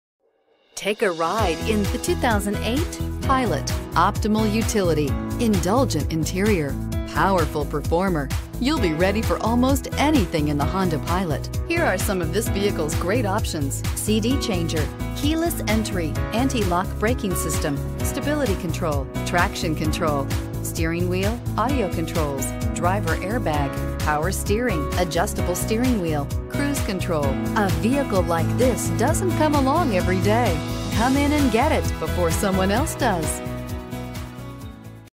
2008 Honda Pilot 58496A
2008 HONDA PILOT Butler, NJ
Stock# 58496A

1301 Rt. 23 South

Delivers 20 Highway MPG and 15 City MPG! This Honda Pilot delivers a Gas V6 3.5L/212 engine powering this Automatic transmission. Vehicle Stability Assist (VSA) w/traction control, Variable Torque Management (VTM-4) 4-wheel drive system, Variable pwr rack & pinion steering.

*This Honda Pilot Comes Equipped with These Options *
Variable intermittent windshield wipers, Upper/lower front row seatback pockets, Tire pressure monitoring system, Synchronized automatic tri-zone climate control system, Sunglasses holder, Steering wheel-mounted audio controls, Sliding sunvisor extensions, Side-impact door beams, Side marker lights, Side curtain airbags on all rows w/rollover sensor.

Come in for a quick visit at Route 23 Automall, 1301 Rt. 23 South, Butler, NJ 07405 to claim your Honda Pilot!